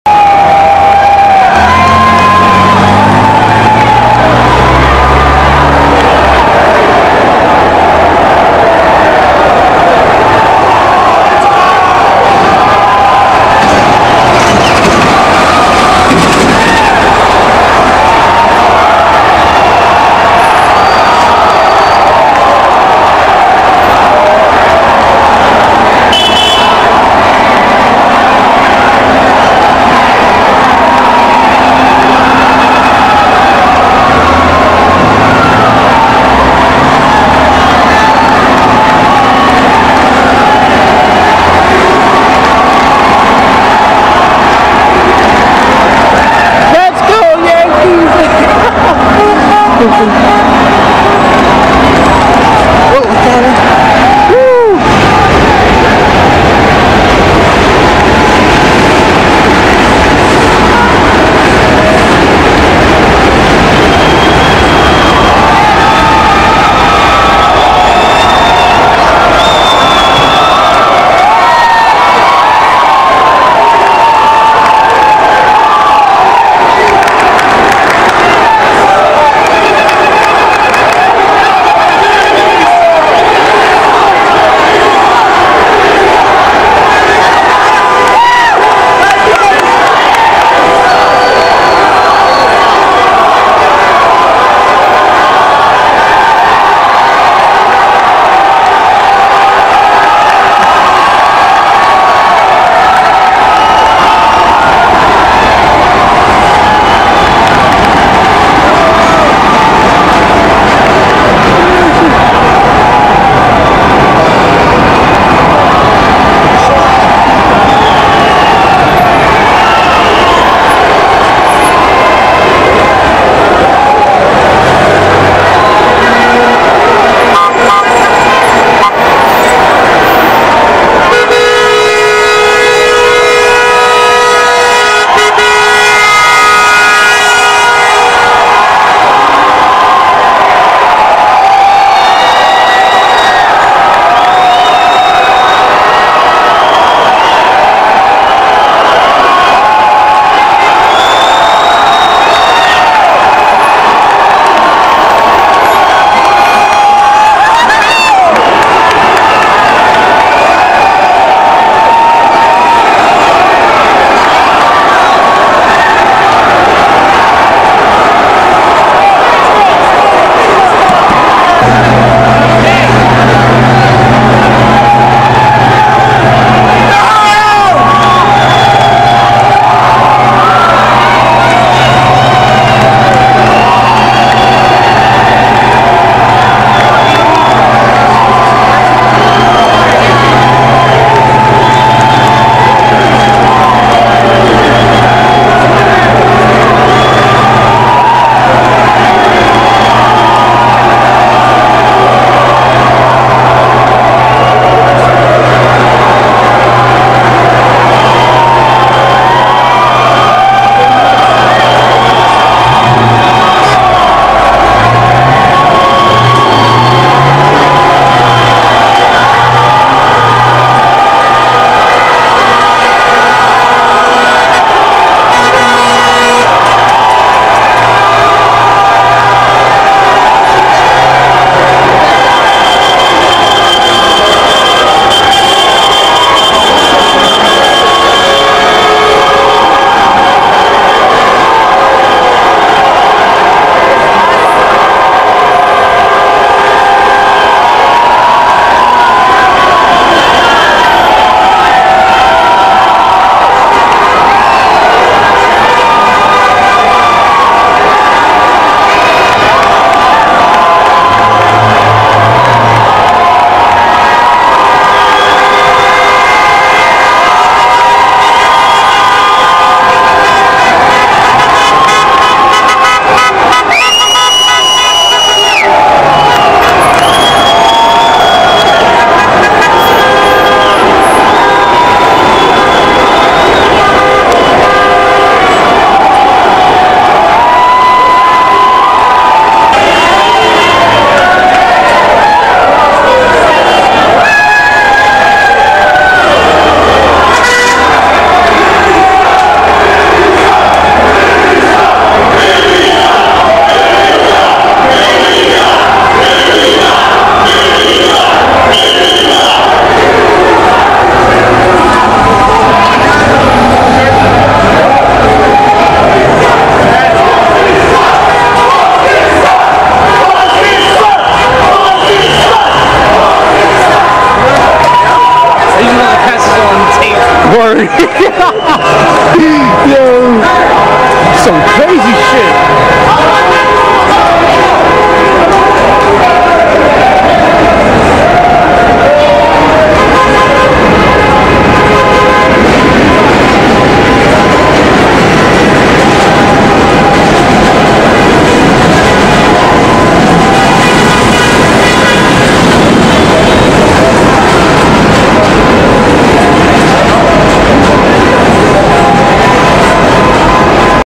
2009 World Series Game 4 Aftermath
They let the Yankees fans in free into Yankee Stadium to watch the game on the big screen. It was great! Our home team whipped the Frillies 7-4 last night and this is what happened! Crazy night! This secton of the Bronx just went buckwild!!